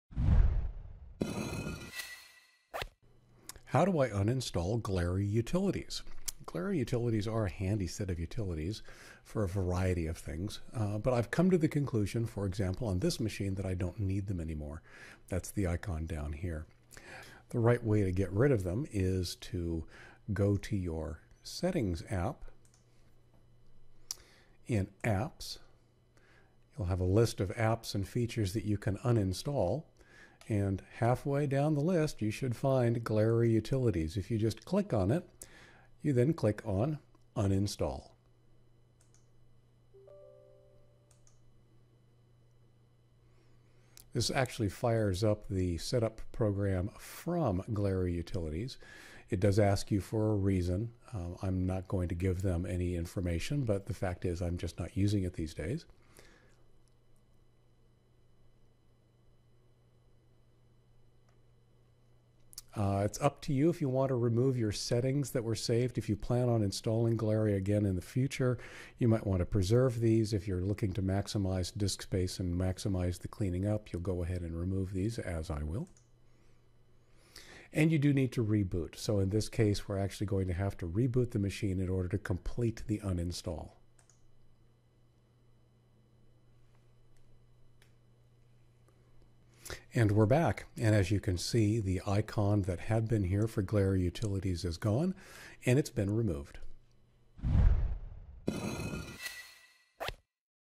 Uninstall Glary Utilities
An Ask Leo! Short. Glary Utilities are a handy set of utilities. If you don't need them any more, I'll show you the right way to uninstall them.

for more questions and answers about technology and making technology work for you.